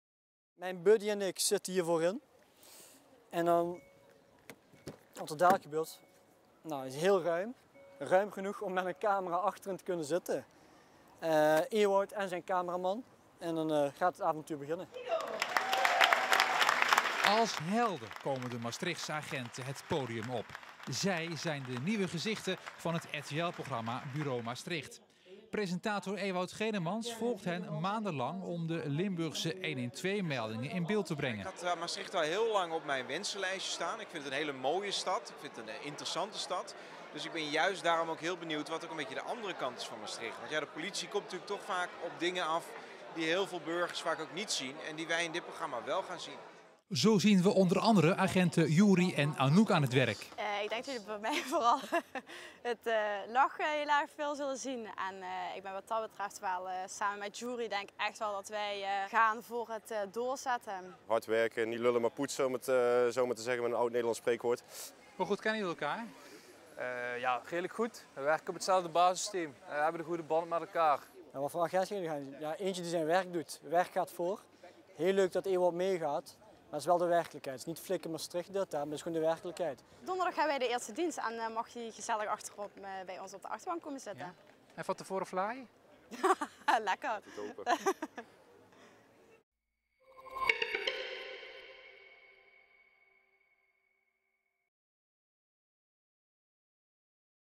Ewout Genemans vanaf deze week de straat op in Maastricht 🚨👮 | L1 Nieuws
Het zesde seizoen van de Bureau-reeks van Ewout Genemans wordt dit jaar in Maastricht opgenomen.